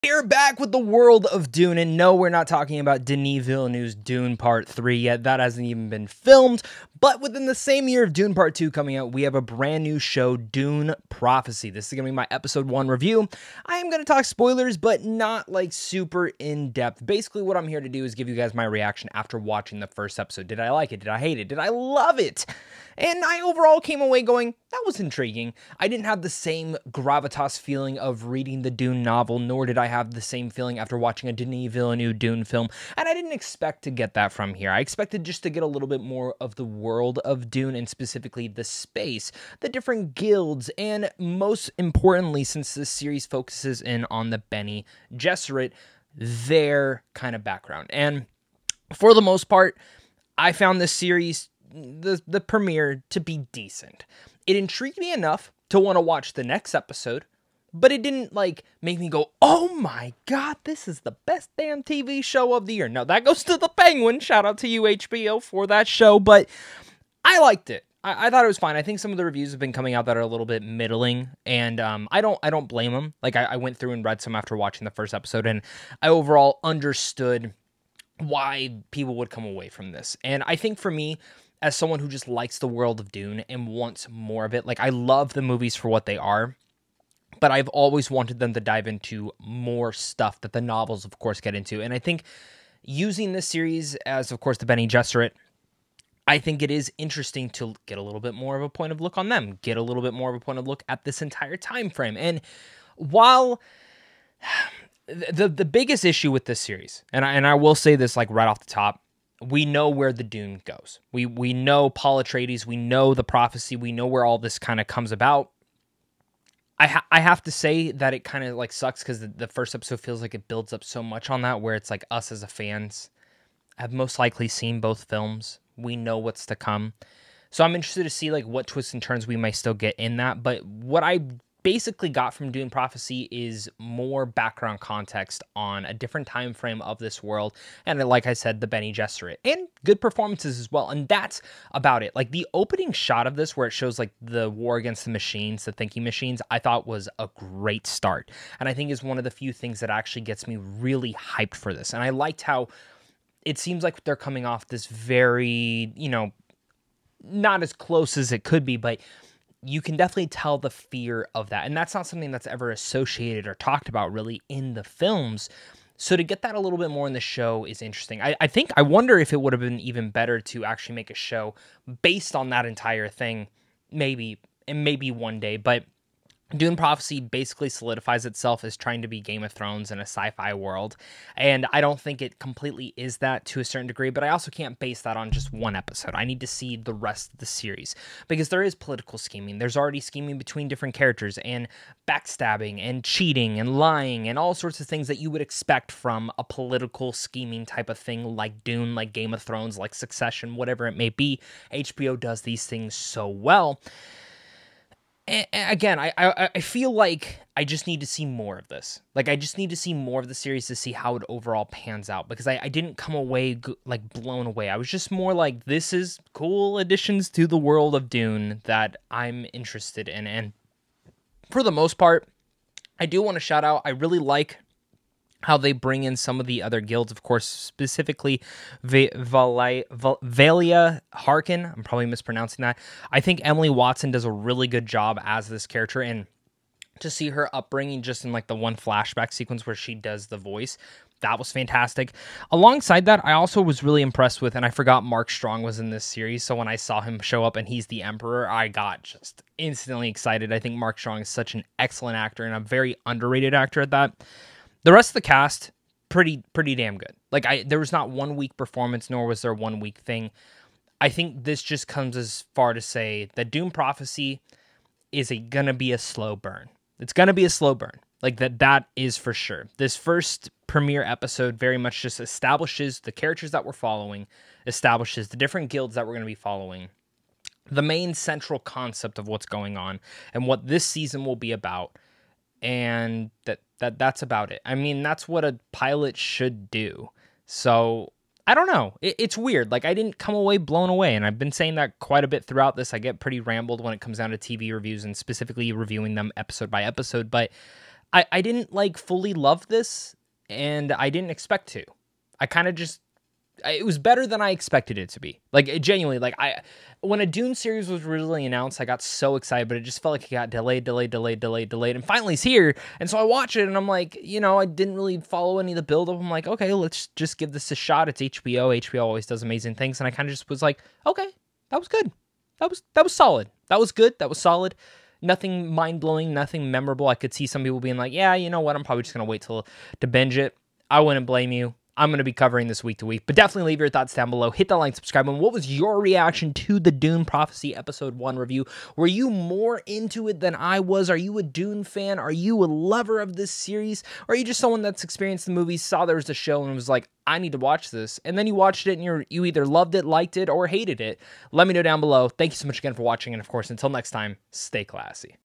Dune: Prophecy Episode 1 Review | HBO MAX | Better than Expected
Dune Prophecy is here with Episode 1 and here is my spoiler review for the brand new Dune Show! Which after its inital reviews weren't the best... but honestly better than expected even it didnt blow me away! Dune Prophecy season 1 episode 1 review!

Set 10,000 years before the events of Dune, the series "follows sisters Valya and Tula Harkonnen as they combat forces that threaten the future of humanity, and establish the fabled sect known as the Bene Gesserit

Cast and Crew: Based on
Sisterhood of Dune
by Brian Herbert
Kevin J. Anderson
Dune
by Frank Herbert
Developed by
Diane Ademu-John
Alison Schapker
Showrunner Alison Schapker
Starring
Emily Watson
Olivia Williams
Travis Fimmel
Mark Strong
Jade Anouka
Chris Mason
Jodhi May
Josh Heuston
Tabu
Charithra Chandran

Find Me Elsewhere in the Multiverse:
Twitter(X): @popetheking
Podcast: Into the Geekverse with Zach Pope on Spotify + Apple